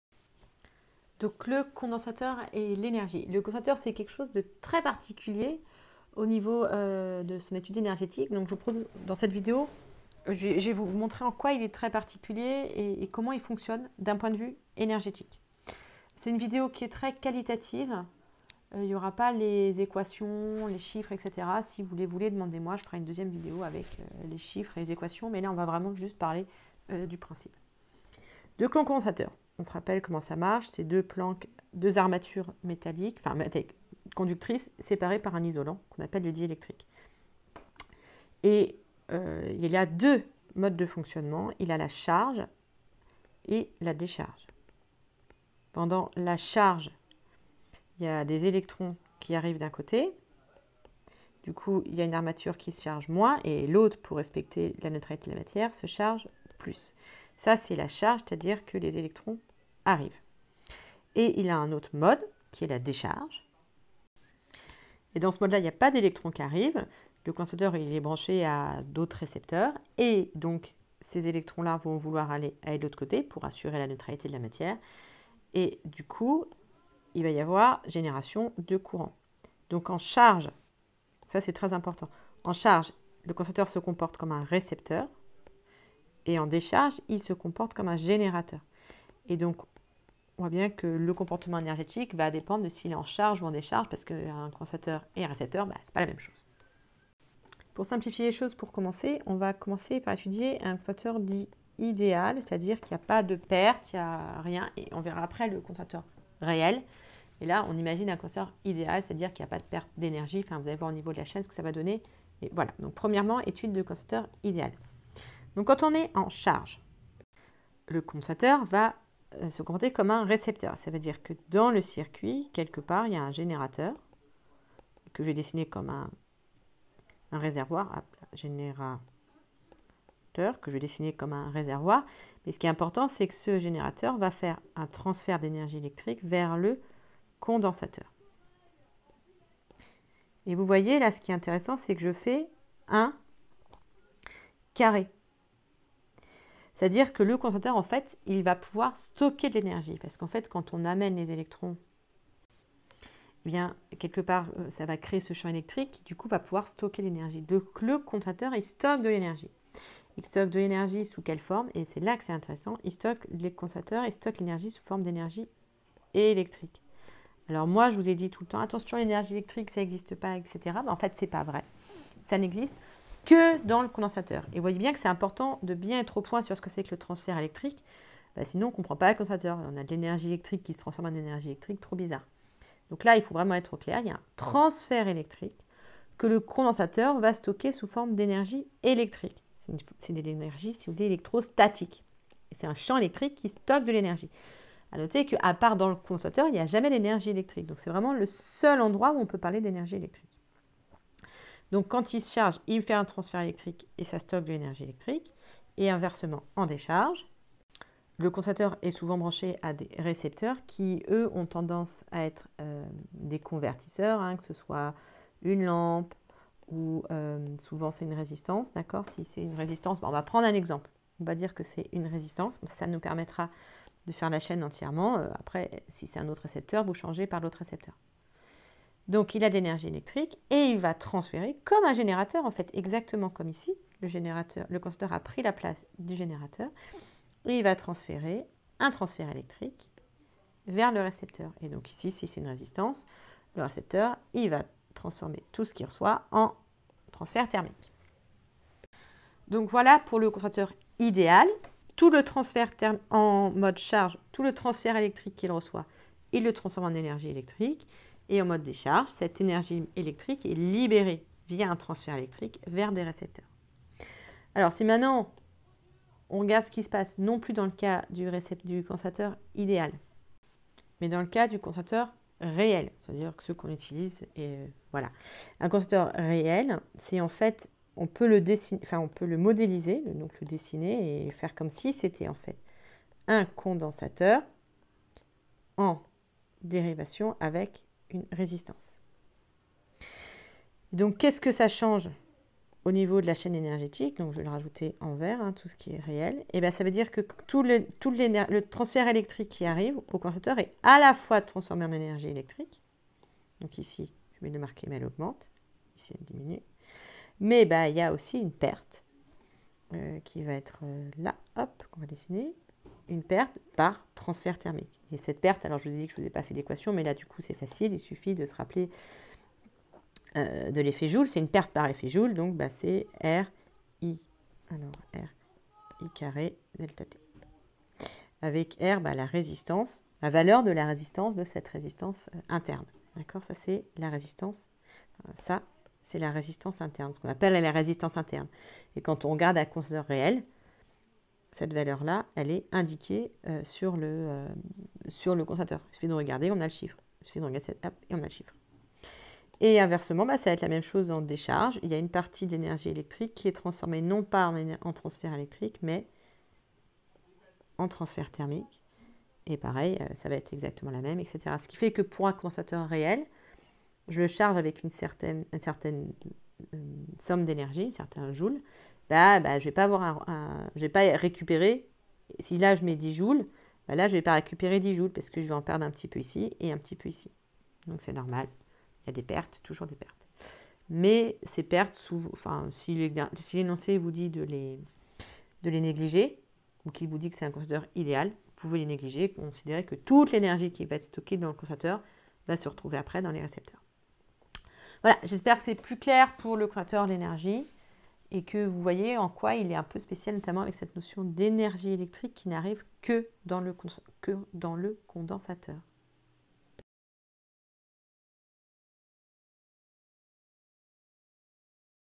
Condensateur et energie
Car le condensateur est un dipôle un peu spécial sur le plan de l'énergie voici une vidéo qui détaille tout.